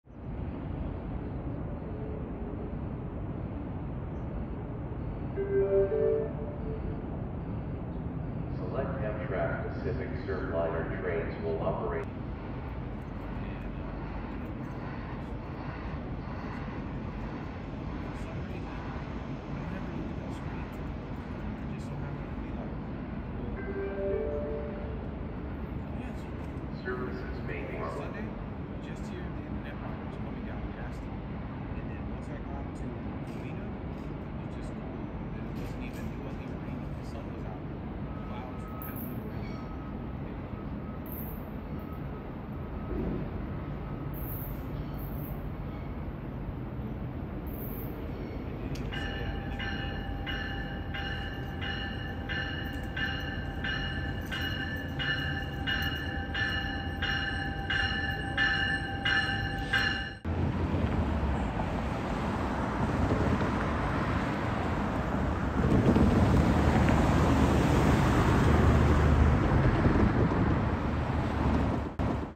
SAN BERNARDINO DEPOT TO COVINA METROLINK STATION
TOOK THE METROLINK FROM SAN BERNARDINO TO COVINA